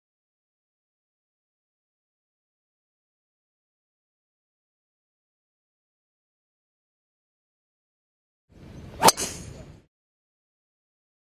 Stock Footage - Golf Ball Kick | VideoHive
Download Template Stock Footage - Golf Ball Kick | VideoHive

Blow to the golf ball. Shot putter. Sound included.